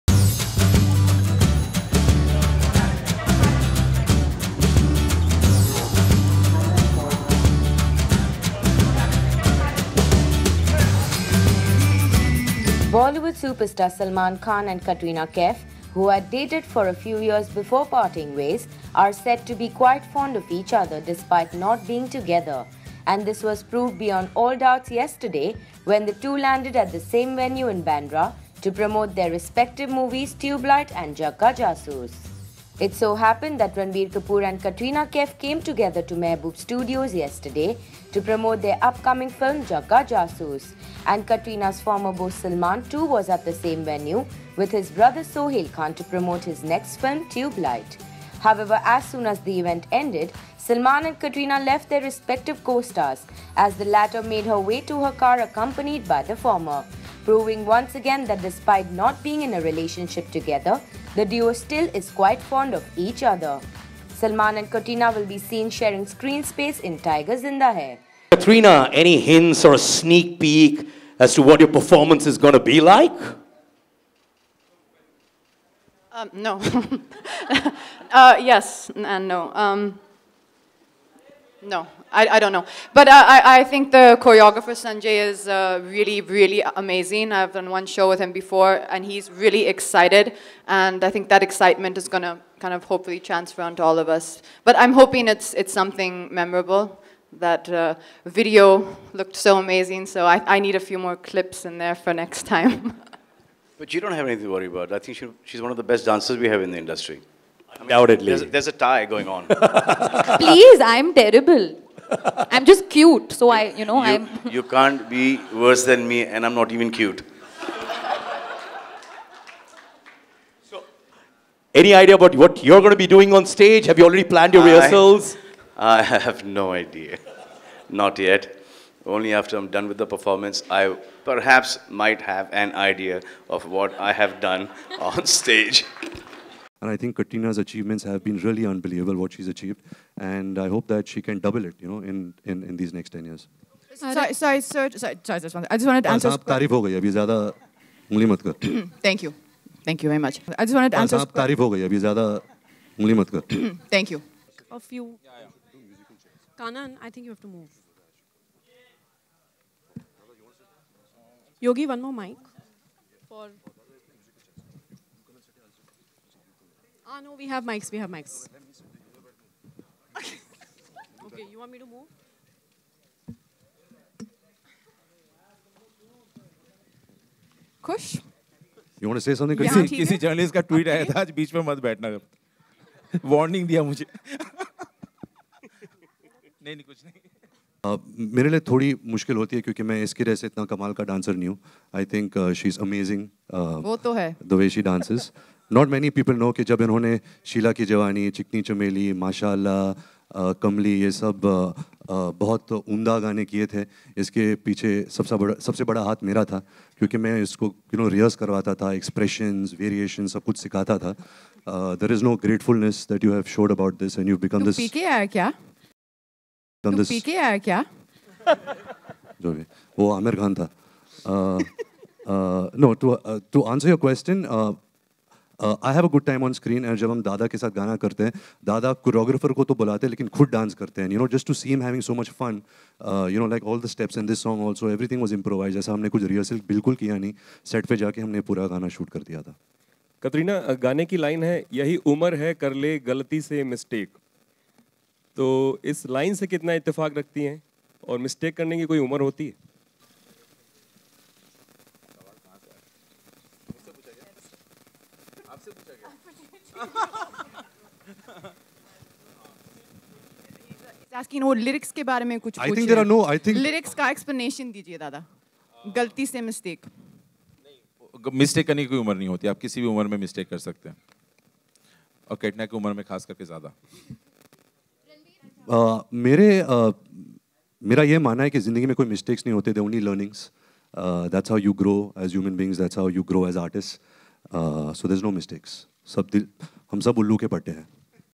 Katrina Leaves Ranbir Kapoor & HUGS Salman Khan
Bollywood superstar Salman Khan and Katrina Kaif, who had dated for a few years before parting ways, are said to be quite fond of each other despite not being together and this was proved beyond all doubts yesterday when the two landed at the same venue in Bandra to promote their respective movies ‘Tubelight’ and ‘Jagga Jasoos’.

It so happened that Ranbir Kapoor and Katrina Kaif came together to Mehboob Studios yesterday to promote their upcoming film ‘Jagga Jasoos’ and Katrina’s former beau Salman too was at the same venue with his brother Sohail Khan to promote his next film ‘Tubelight’. However, as soon as the event ended, Salman and Katrina left their respective co-stars as the latter made her way to her car accompanied by the former, proving once again that despite not being in a relationship together, the duo still are quite fond of each other.

Salman and Katrina will be seen sharing screen space in ‘Tiger Zinda Hai’.